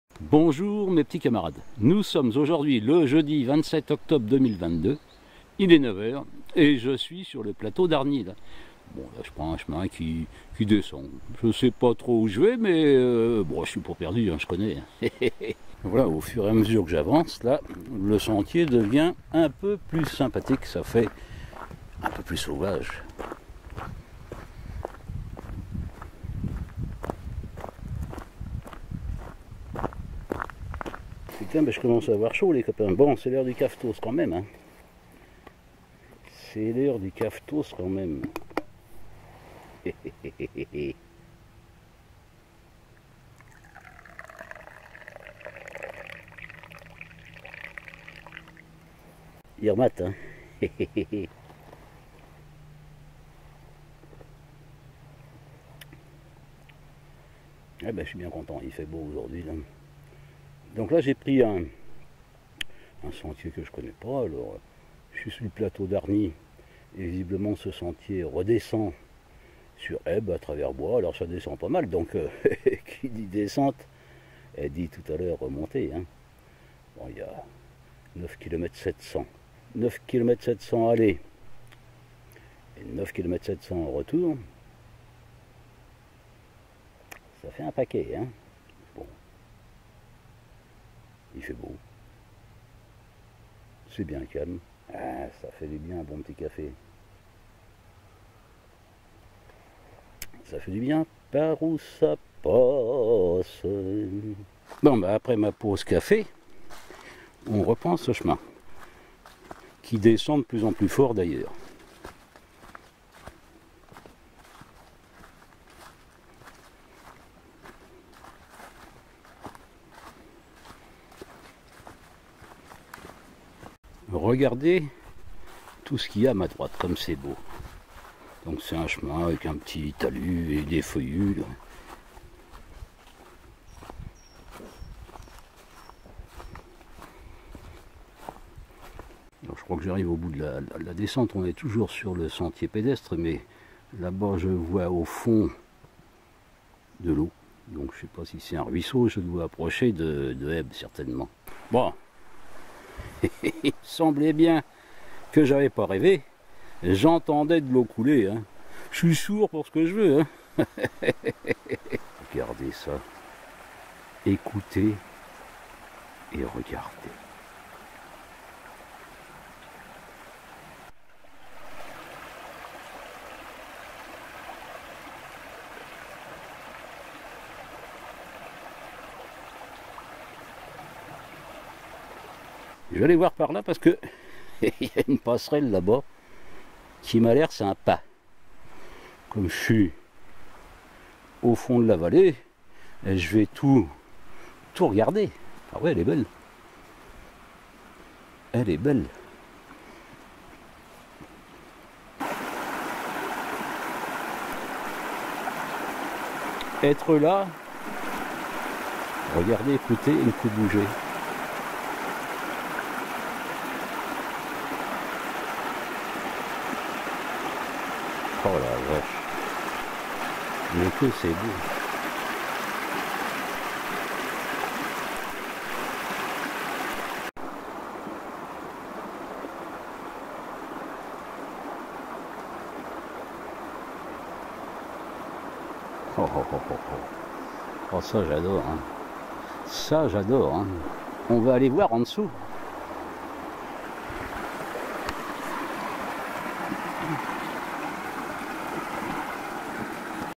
Hargnies Haybes Bois du Laurier
Rando sur les hauteurs d'Hargnies en direction de Haybes, par les Bois du Laurier.